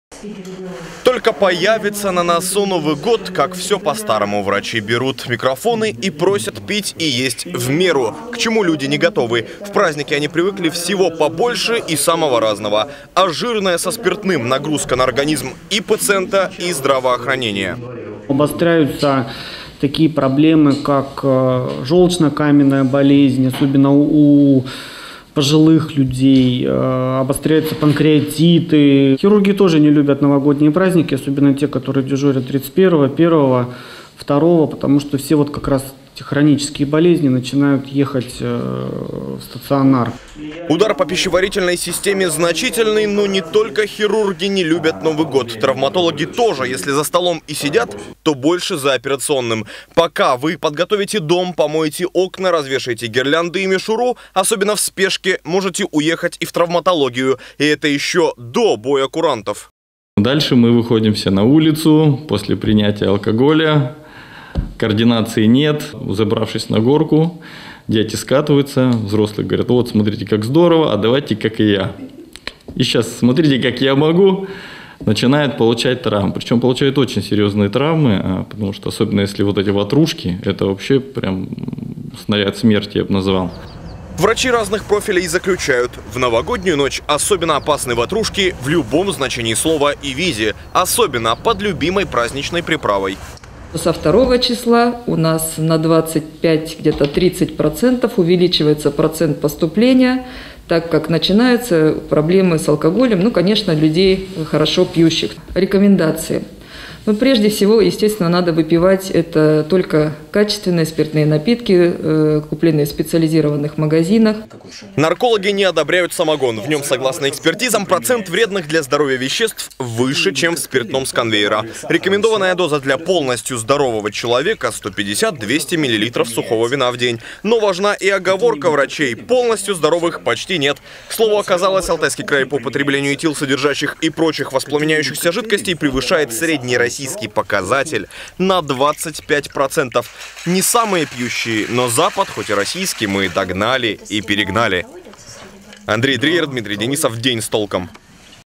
Отравления и переломы: как встретить Новый год без последствий
Новый год – праздник  семьи, а не живота. Это уже в который год с середины декабря начинают твердить врачи. Почему алкоголь – зло, а снежные горки – не для взрослых?